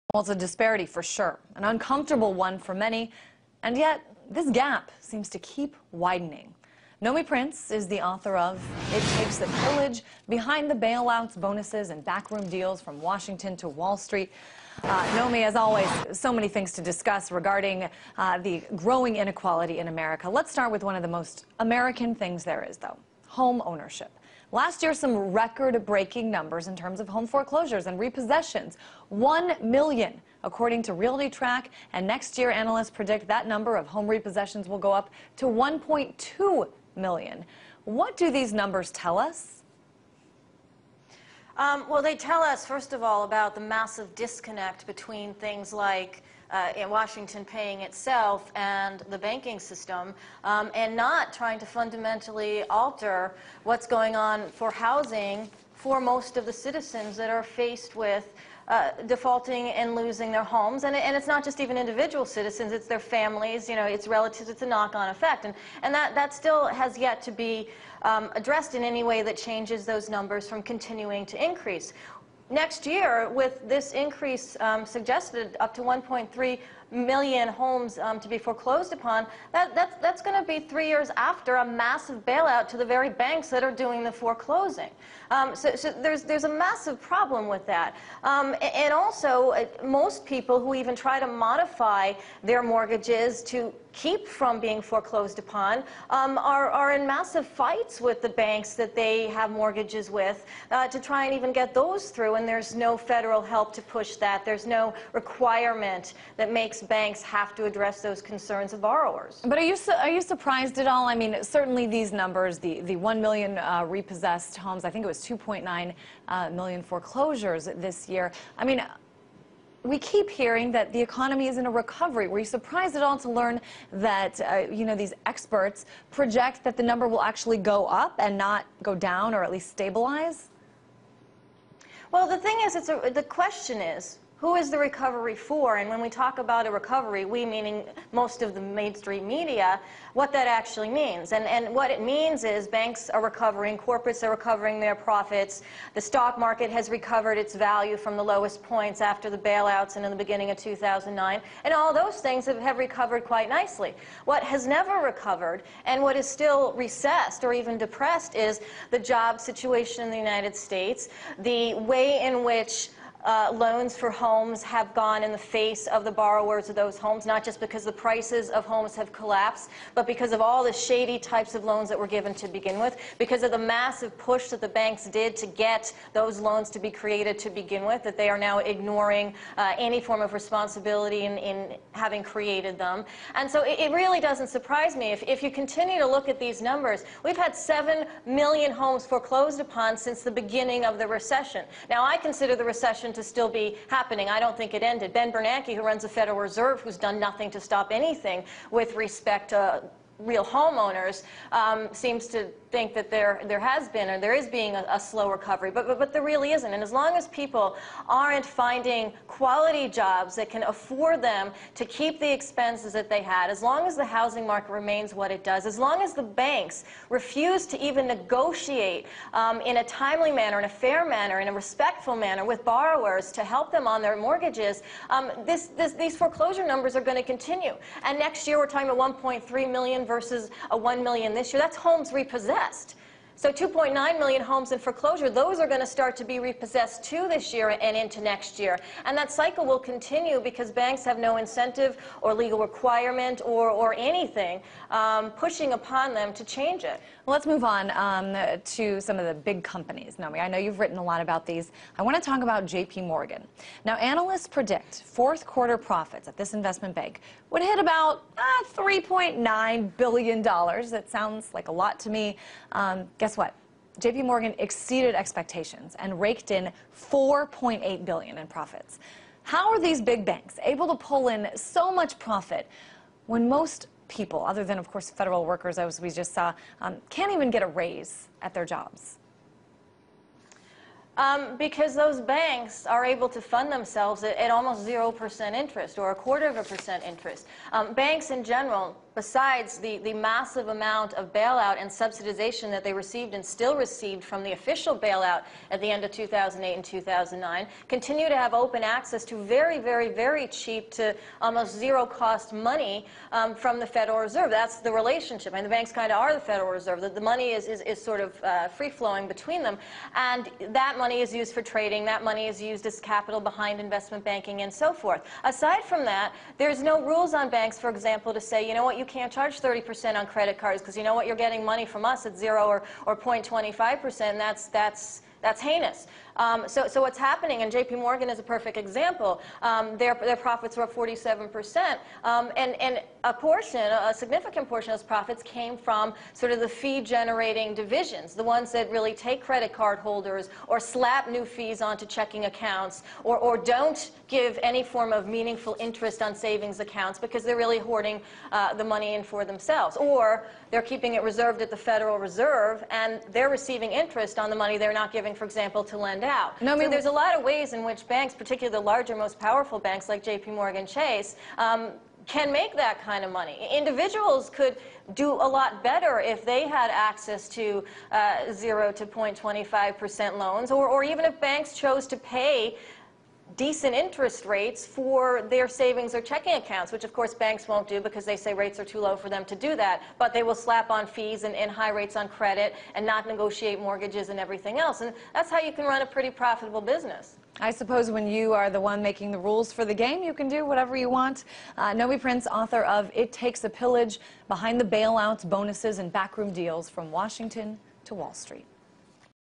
Demos' Nomi Prins - Wall Street vs. Main Street
Demos' Nomi Prins criticizes the jobless "recovery" from the Great Recession. She exposes the unfair support that Wall Street corporations enjoy from the federal government while working class Americans continue to struggle.